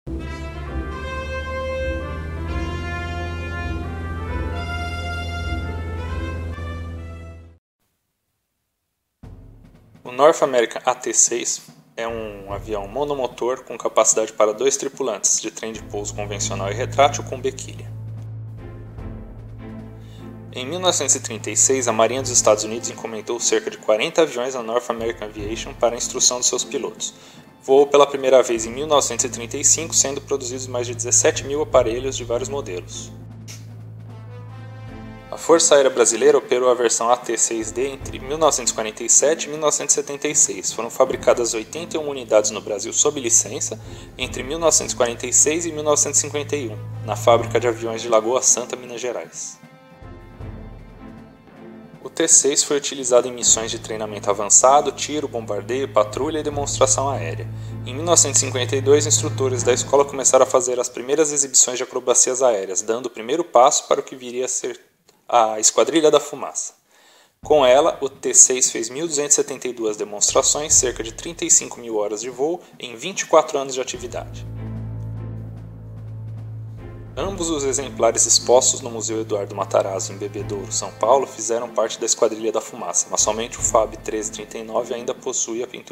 T-6 Texan da Esquadrilha da Fumaça - Caçadores da Aeronave Perdida Ep #11
Episódio 11 da série "Caçadores da Aeronave Perdida", que busca o destino de aeronaves que não voam mais. Nesse episódio, um North American T-6 Texan que pertenceu à FAB exposto no Museu Eduardo Matarazzo, em Bebedouro/SP.